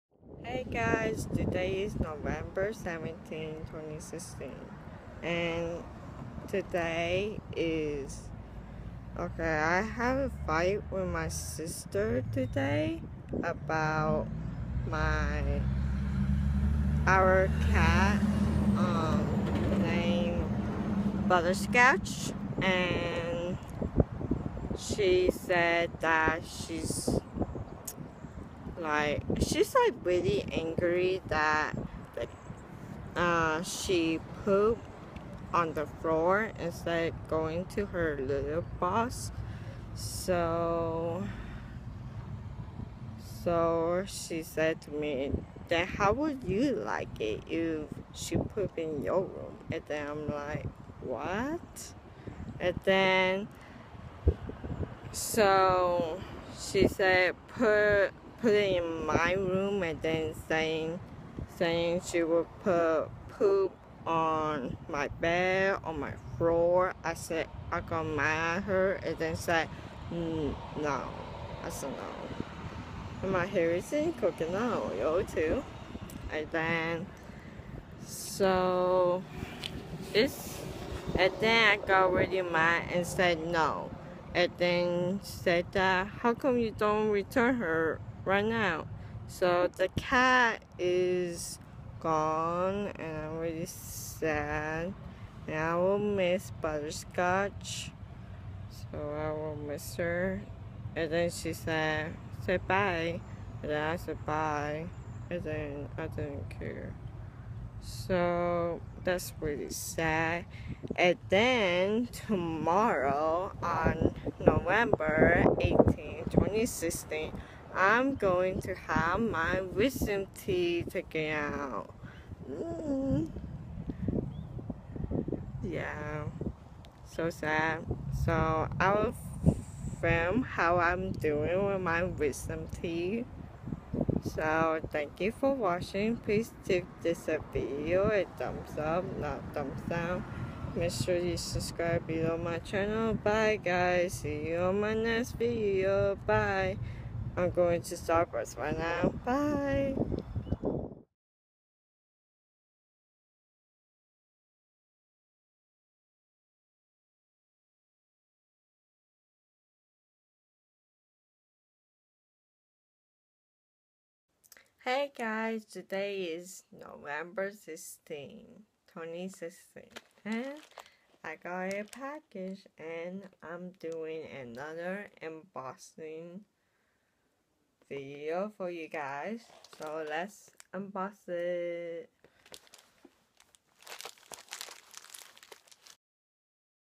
Talk About Butterscotch, Wisdom Teeth, and going to Starbucks-November 17,2016
In this video:
I have some BAD NEWS for you guys:(  I'm getting Wisdom teeth out and I was NOT ready. I went to starbucks😍   There are 3 picture in this video👌🏻
🚫Please NO hate or hate comments on here🚫  Thank you😊